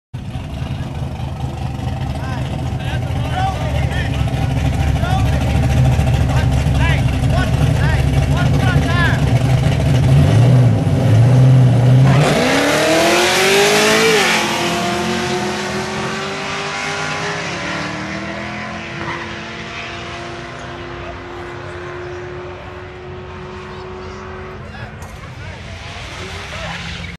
MadTunes Racing TransAm 10.8 second pass.wmv
Ran back to back 10.88s  at 123 mph on this day in about 55% humidity.  The car is a 404 stroker with a race weight of around 3800 lbs +